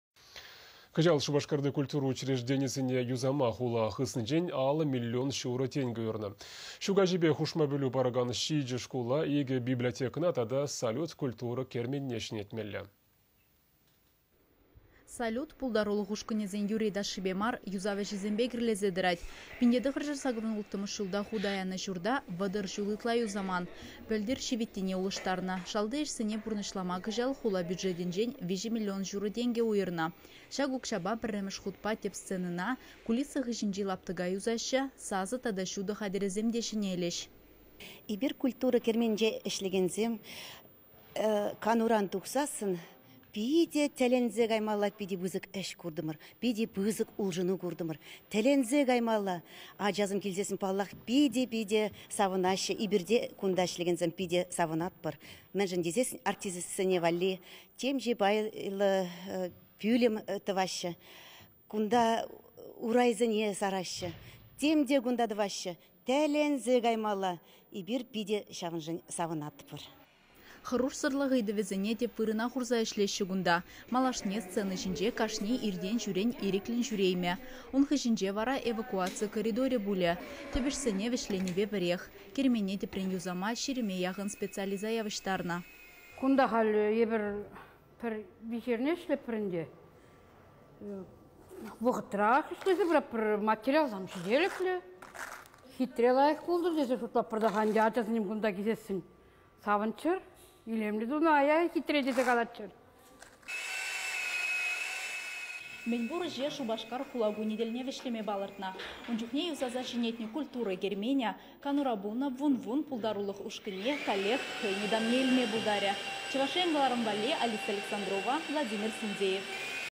Шупашкарти культура учрежденийĕсене юсама хула хыснинчен 50 миллион çурă тенкĕ уйăрнă
Çу каçипе хушма пĕлÿ паракан 7 шкула, 2 библиотекăна тата «Салют» культура керменне çĕнетмелле.